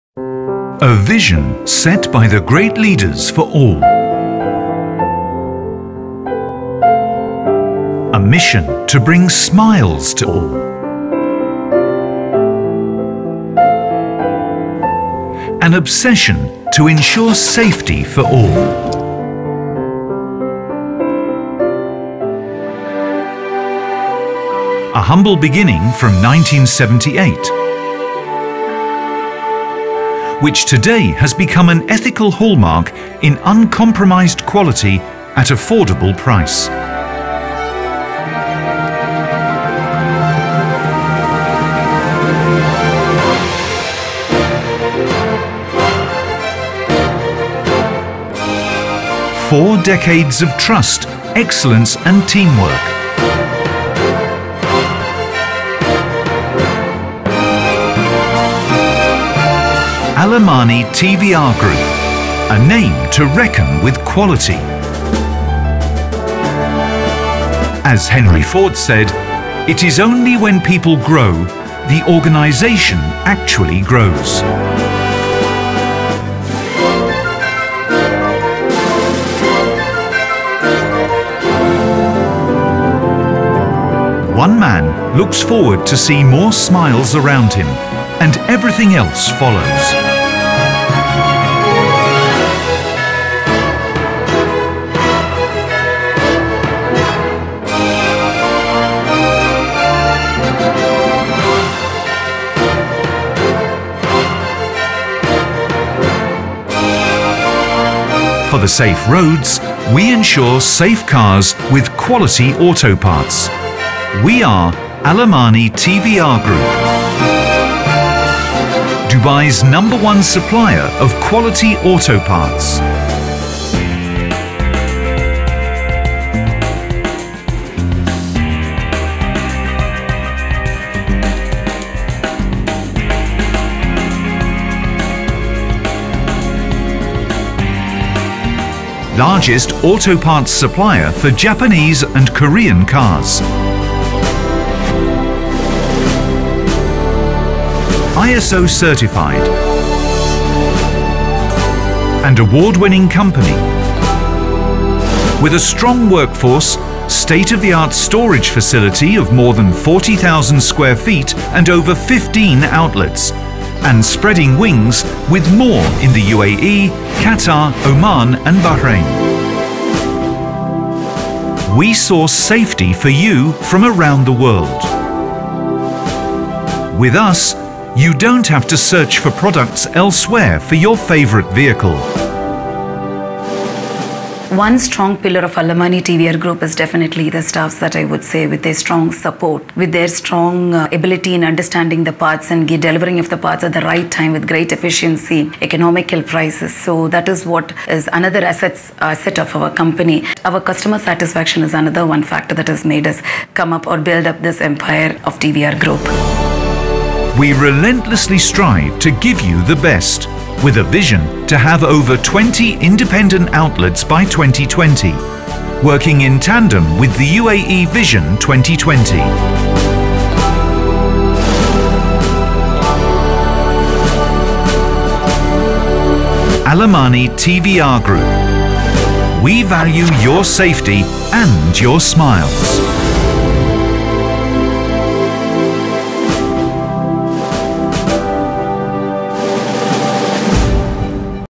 Al Amani TVR Group Corporate Video
Celebrating 40 years  - Leading auto parts dealer in the UAE,Qatar,Oman and Bahrain